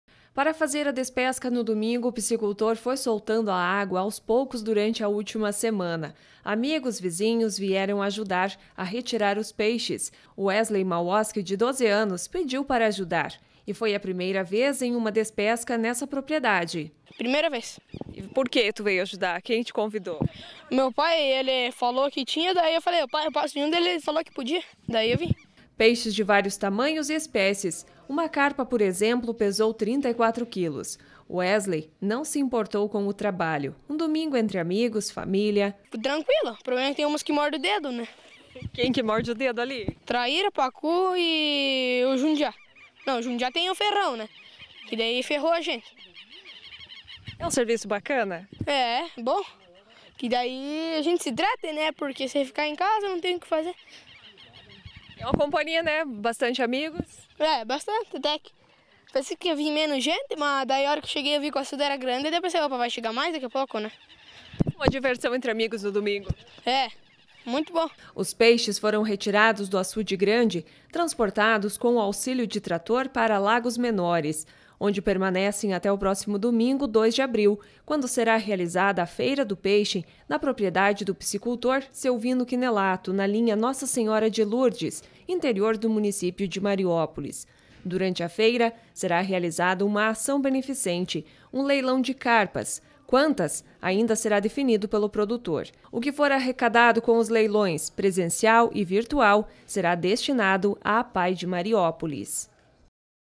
Piscicultor de Mariópolis se prepara pra feira do peixe e ação solidária
Atualidade: Piscicultor organiza feira do peixe na propriedade em Mariópolis. E durante o evento realizara leilão de carpas em prol da APAE do município.